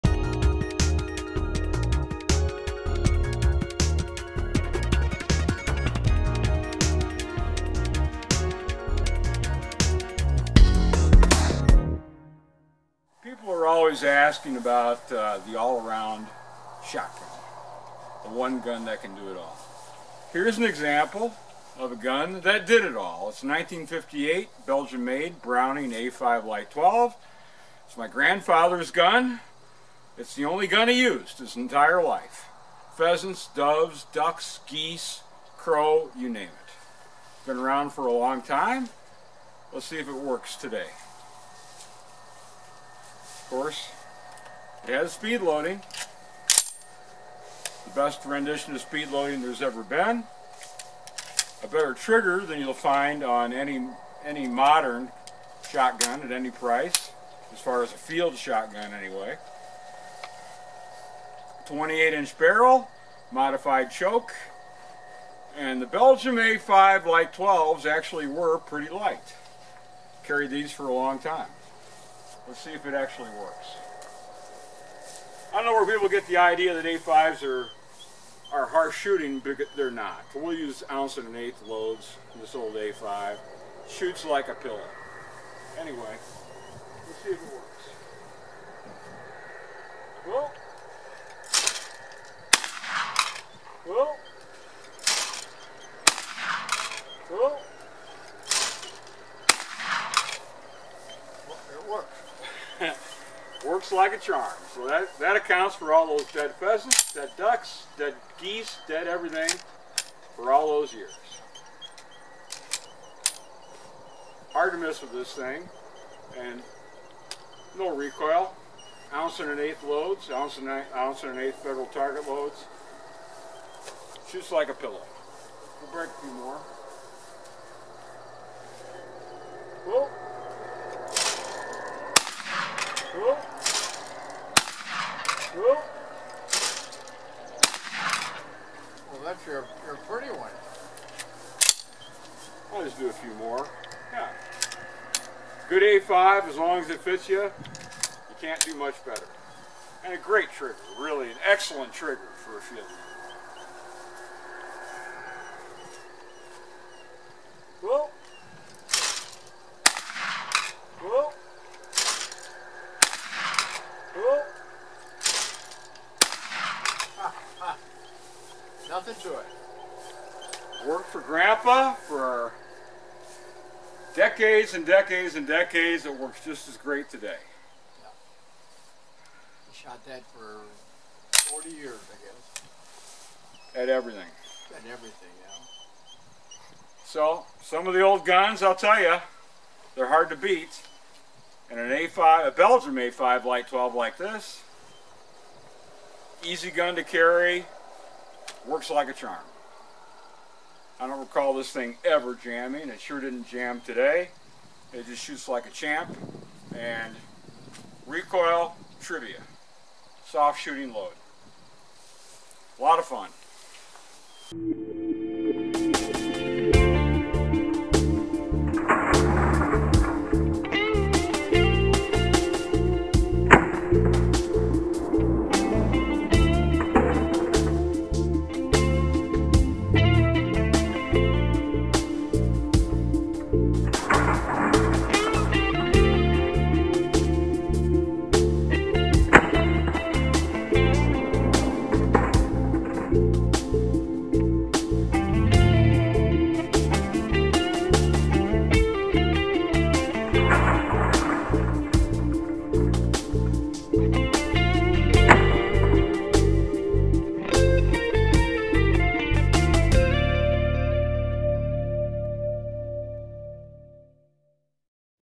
Browning A-5 Light Twelve: 55 Years Later
Few shotguns are truly "lifetime guns," but this Browning Automatic-Five is. Few shotguns have "done it all," but this Browning A-5 has. Few shotguns function as flawlessly over 55 years after they are manufactured . . . but this 1958 Belgium Browning sure does. It is the only firearm my grandfather ever owned, and it was hunted hard every year. It shoots as well today as it did when it was brand new. From randywakeman.com.